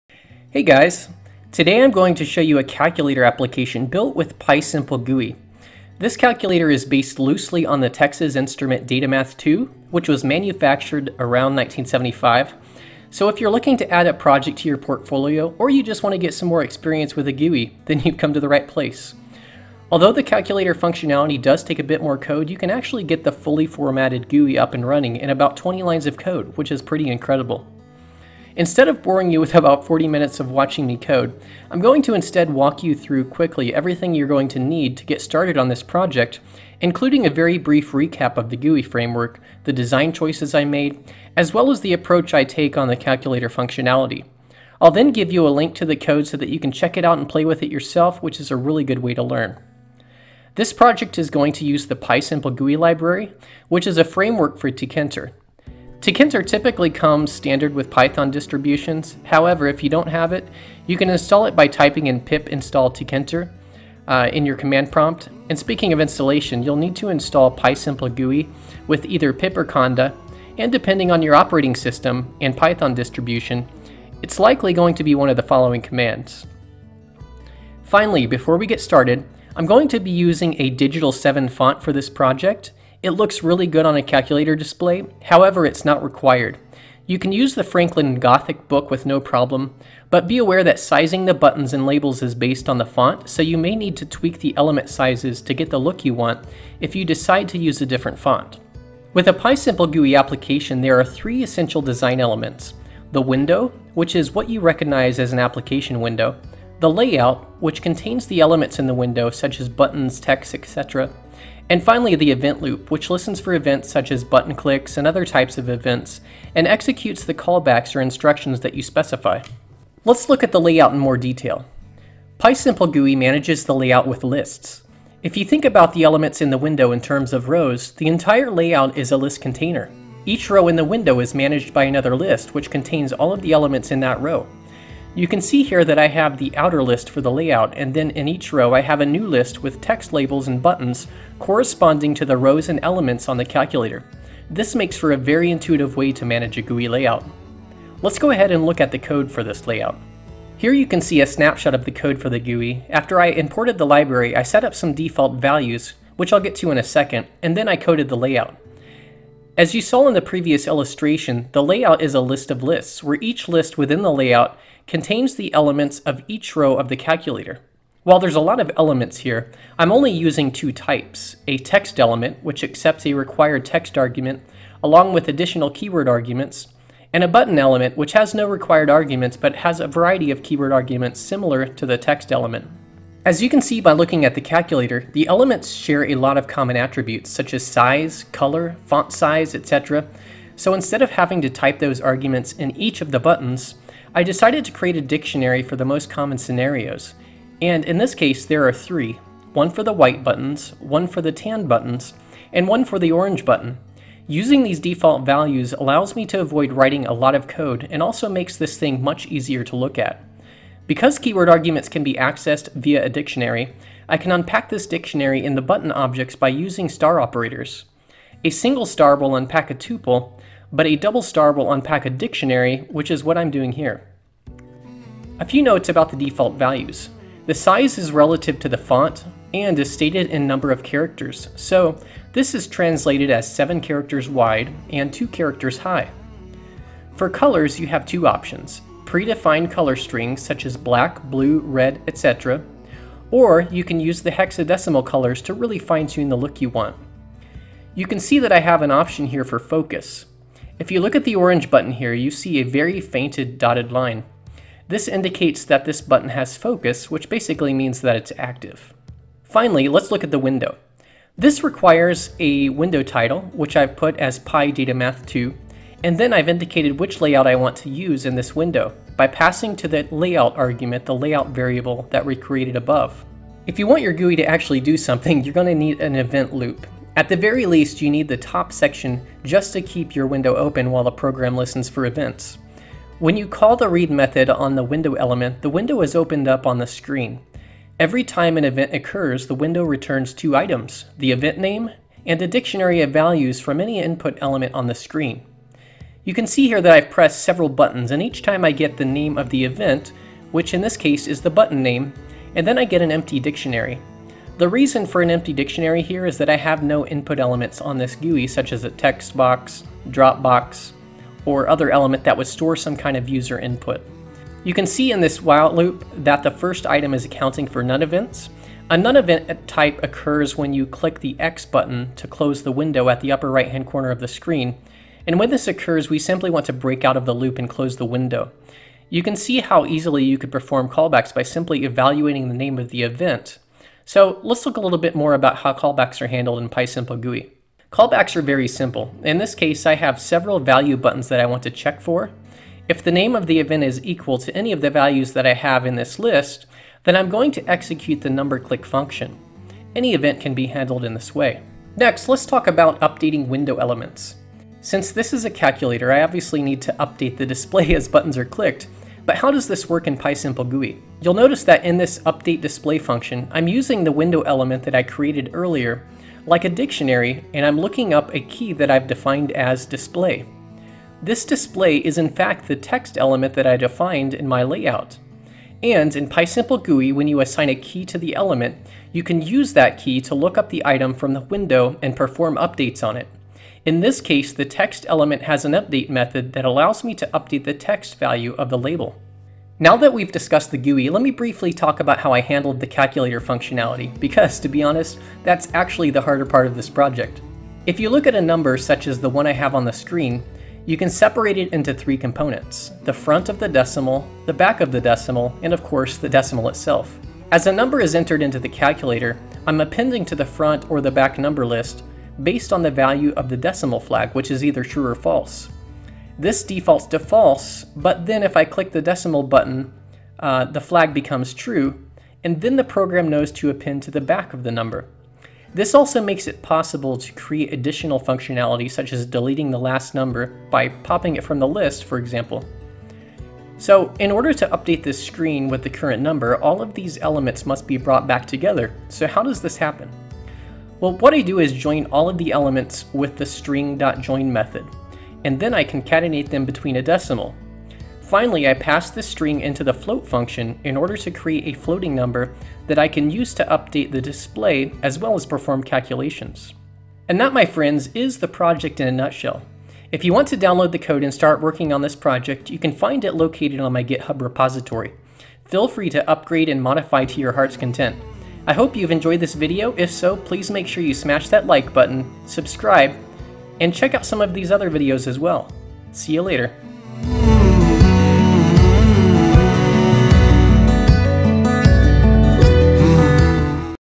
Python Calculator with GUI | PySimpleGUI | Texas Instruments DataMath II
In this video, learn how to build a Python Calculator with GUI using the PySimpleGUI library. To add some style, I modeled this after the Texas Instruments DataMath II, which was manufactured around 1975. This is a great project to flex your GUI skills if you're just getting started, and also provides a challenge on the calculator functionality.

In this video:
0:00 | overview
0:52 | required libraries
1:18 | digital 7 font (link)
1:38 | design elements
2:00 | the layout
2:41 | code view of layout
3:03 | sharing common attributes
3:36 | notes on default settings
4:19 | the window element
4:33 | the event loop
5:35 | using callbacks
5:51 | updating the display
6:30 | my approach to the calc functions